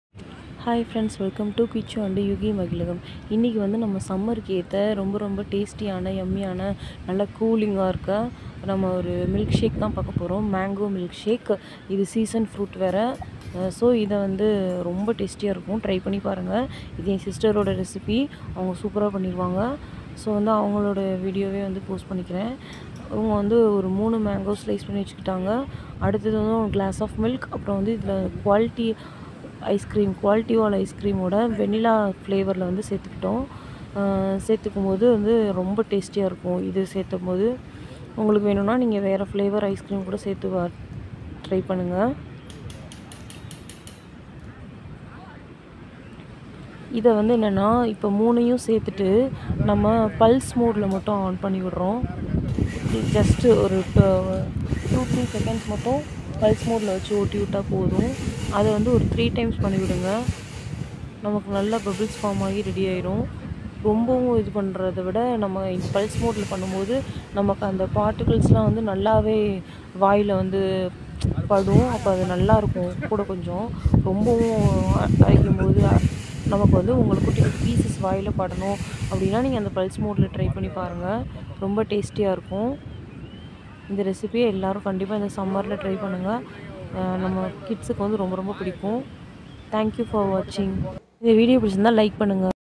Simple mango milkshake/கோடைக்காலத்தில் சில்லுனு மெங்கோ மில்க்ஷேக்
Ingredients
mango -3nos
milk -1glass
vanilla icecream -100g
add some icecream again as topping it gives amazing taste.